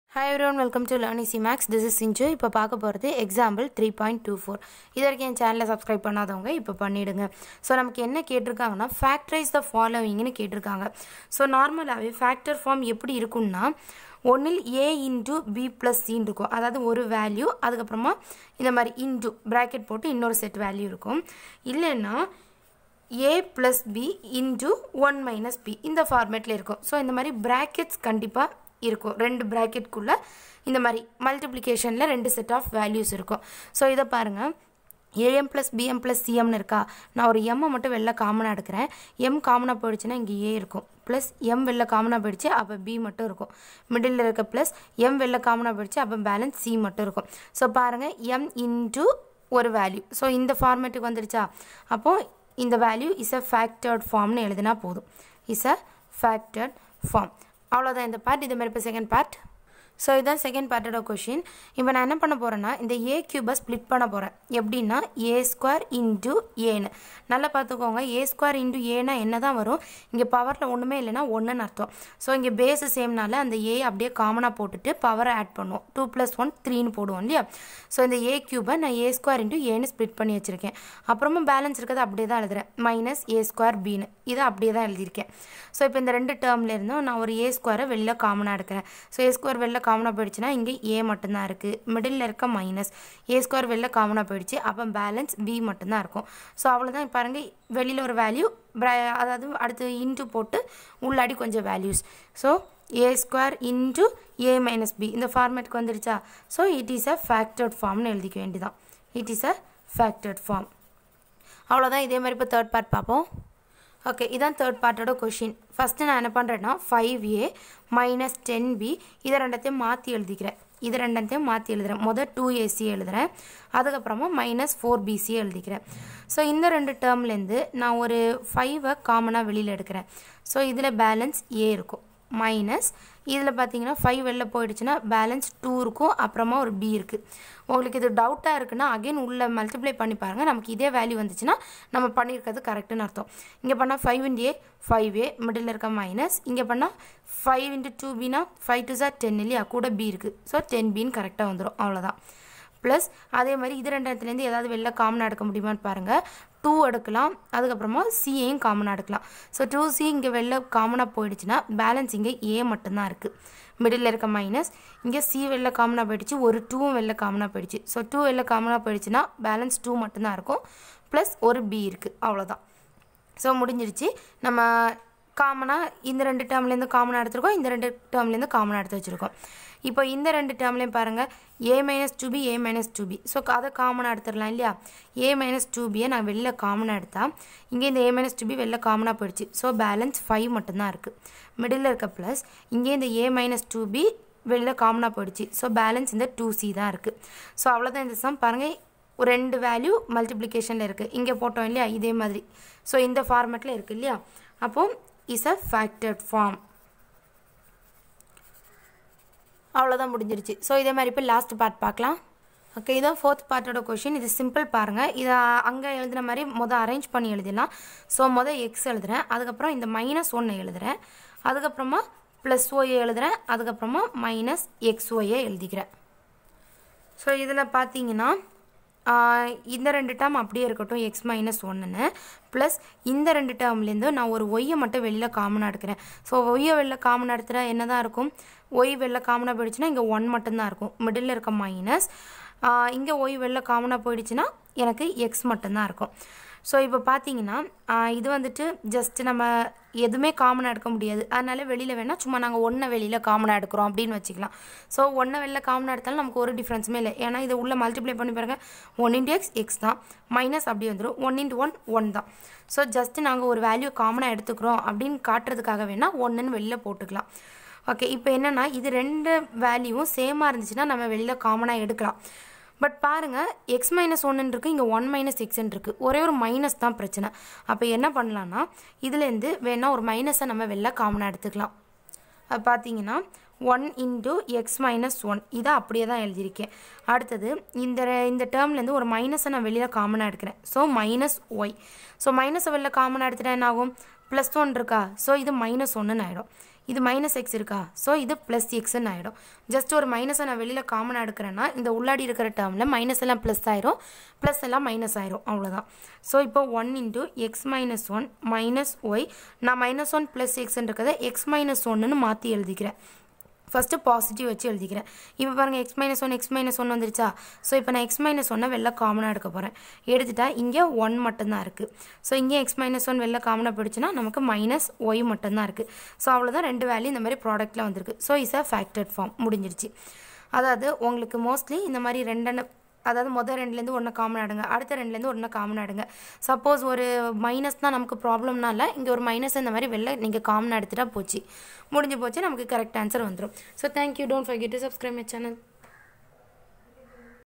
9th Maths Chapter 3 Example 3.24 Samacheer State board Tamil English medium
Hello Everyone this is Sinju. Here I have explained Example 3.24 from Chapter 3 for 9th std. Also I have Explained the problems in a more simple and easy method. Watch the video and then try to solve the problem on your own. It will surely help you. All the Best to score full marks in maths